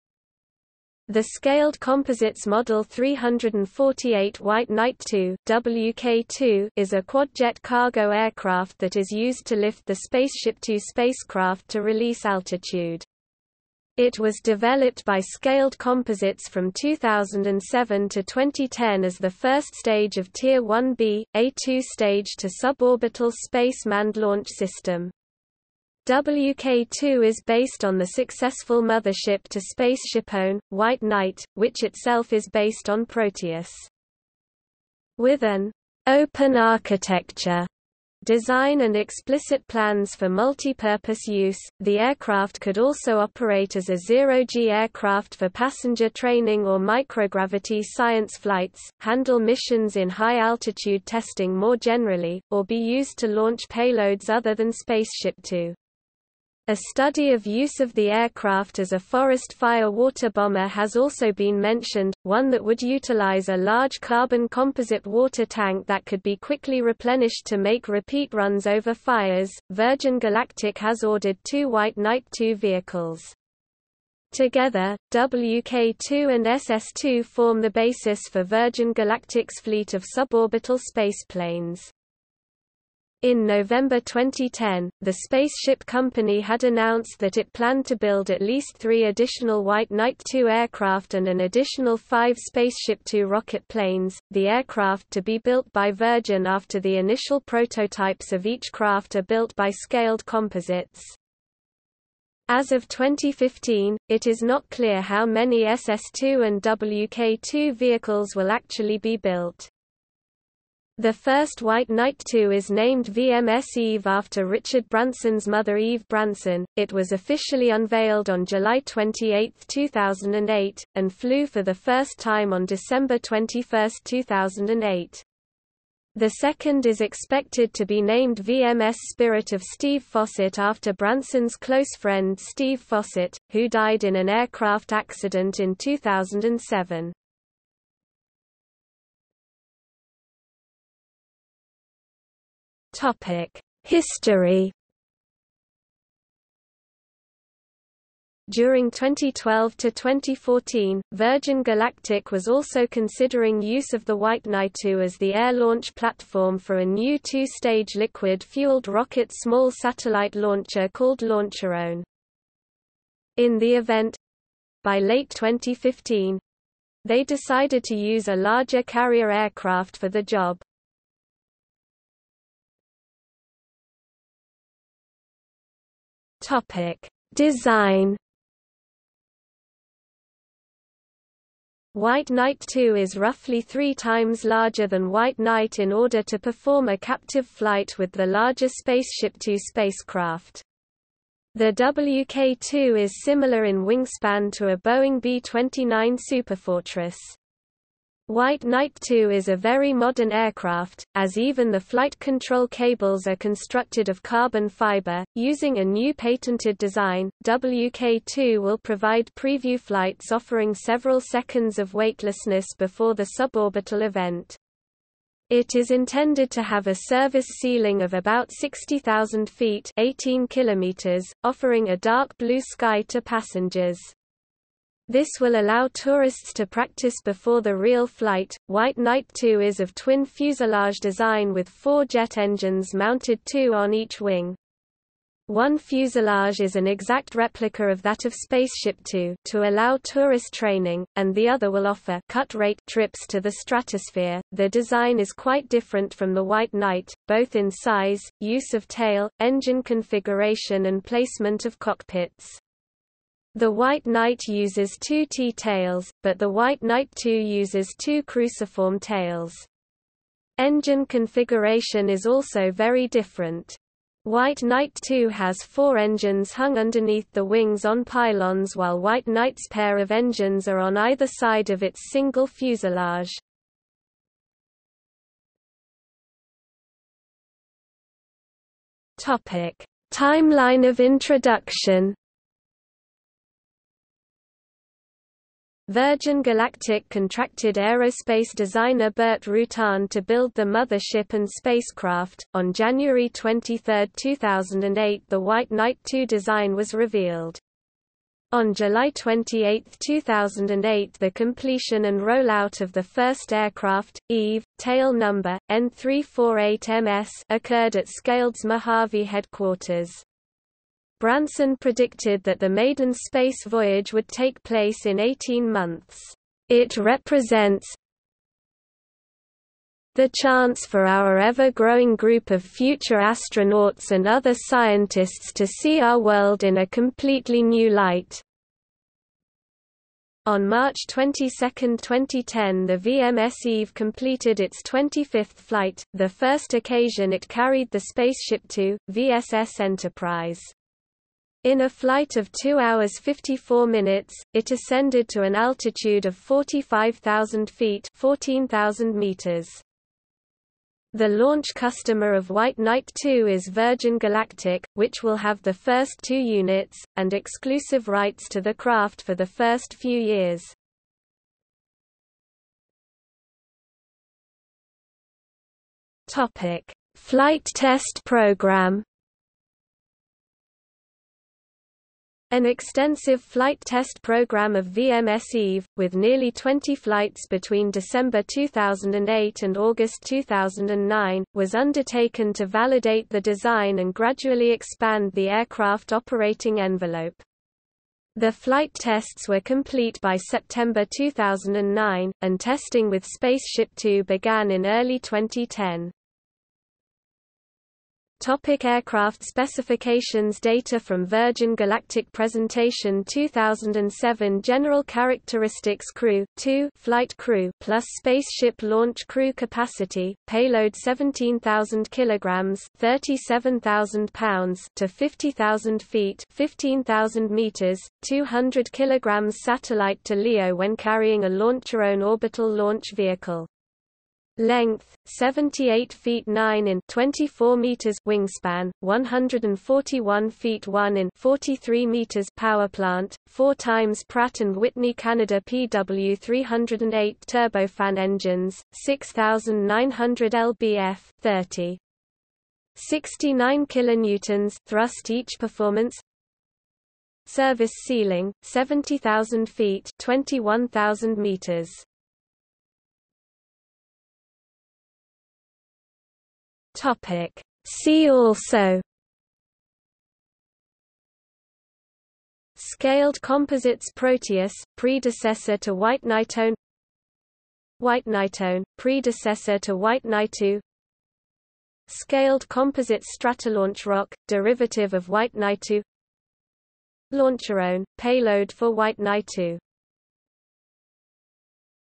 Scaled Composites White Knight Two | Wikipedia audio article
00:02:19 1 History
00:02:52 2 Design
00:04:35 3 Timeline of introduction
00:06:02 3.1 Flight test program
00:06:38 4 Aircraft specifications
00:07:33 5 See also

- Socrates

SUMMARY
The Scaled Composites Model 348 White Knight Two (WK2) is a quadjet cargo aircraft that is used to lift the SpaceShipTwo spacecraft to release altitude. It was developed by Scaled Composites from 2007 to 2010 as the first stage of Tier 1b, a two-stage to suborbital-space manned launch system. WK2 is based on the successful mothership to SpaceShipOne, White Knight, which itself is based on Proteus.
With an "open architecture" design and explicit plans for multi-purpose use, the aircraft could also operate as a zero-g aircraft for passenger training or microgravity science flights, handle missions in high-altitude testing more generally, or be used to launch payloads other than SpaceShipTwo. A study of use of the aircraft as a forest fire water bomber has also been mentioned, one that would utilize a large carbon composite water tank that could be quickly replenished to make repeat runs over fires.Virgin Galactic has ordered two White Knight Two vehicles.  Together, WK2 and SS2 form the basis for Virgin Galactic's fleet of suborbital spaceplanes.
In November 2010, The Spaceship Company had announced that it planned to build at least three additional White Knight Two aircraft and an additional five SpaceShipTwo rocket planes, the aircraft to be built by Virgin after the initial prototypes of each craft are built by Scaled Composites.

As of  2015, it is not clear how many SS2 and WK2 vehicles will actually be built.
The first White Knight Two is named VMS Eve after Richard Branson's mother Eve Branson; it was officially unveiled on July 28, 2008, and flew for the first time on December 21, 2008.  The second is expected to be named VMS Spirit of Steve Fossett after Branson's close friend Steve Fossett, who died in an aircraft accident in 2007.